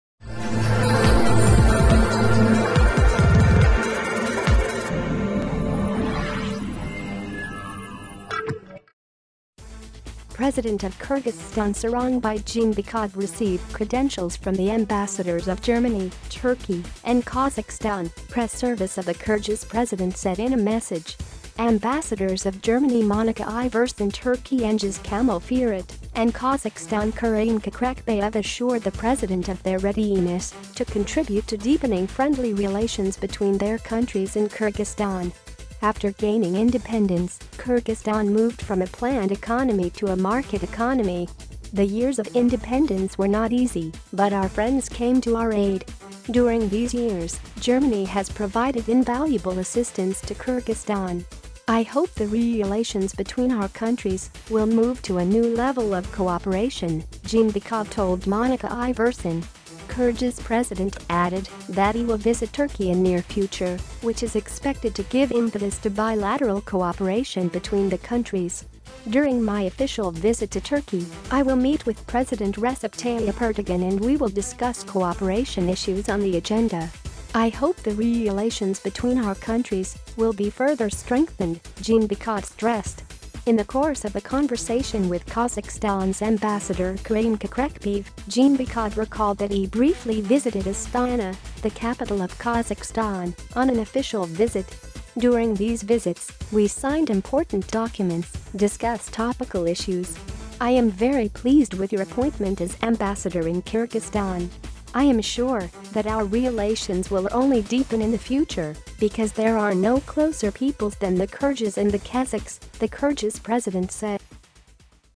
Kyrgyz president receives credentials from ambassadors of Turkey, Kazakhstan, Germany
President of Kyrgyzstan Sooronbai Jeenbekov received credentials from the ambassadors of Germany, Turkey and Kazakhstan, press service of the Kyrgyz president said in a message.

Ambassadors of Germany Monica Iversen, Turkey Сengiz Kamil Firat, and Kazakhstan Karim Kokrekbayev assured the president of their readiness to contribute to deepening friendly relations between their countries and Kyrgyzstan.

"After gaining independence, Kyrgyzstan moved from a planned economy to a market economy. The years of independence were not easy, but our friends came to our aid. During these years, Germany has provided invaluable assistance to Kyrgyzstan. I hope the relations between our countries will move to a new level of cooperation," Jeenbekov told Monica Iversen.

Kyrgyz president added that he will visit Turkey in near future, which is expected to give impetus to bilateral cooperation between the countries.

"During my official visit to Turkey, I will meet with President Recep Tayyip Erdogan and we will discuss cooperation issues on the agenda. I hope the relations between our countries will be further strengthened," Jeenbekov stressed.

In the course of the conversation with Kazakhstan's ambassador Karim Kokrekbaev, Jeenbekov recalled that he briefly visited Astana, the capital of Kazakhstan, on an official visit.

"During these visits, we signed important documents, discussed topical issues. I am very pleased with your appointment as ambassador in Kyrgyzstan. I am sure that our relations will only deepen in the future, because there are no closer peoples than the Kyrgyz and the Kazakhs," the Kyrgyz president said.